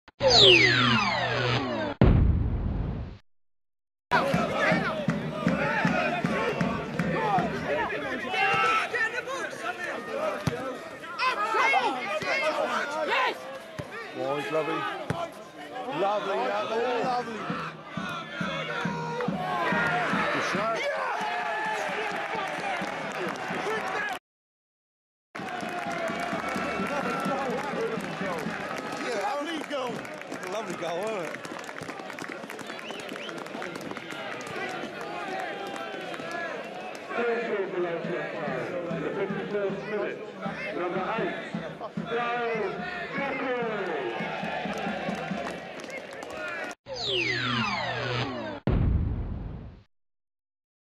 Lowestoft Town vs Billericay goal
Lowestoft Town's only goal in the Ryman Premier fixture against Billericay at Crown Meadow 02/02/11. Scored by Dale Cockrill. Final score 1 all.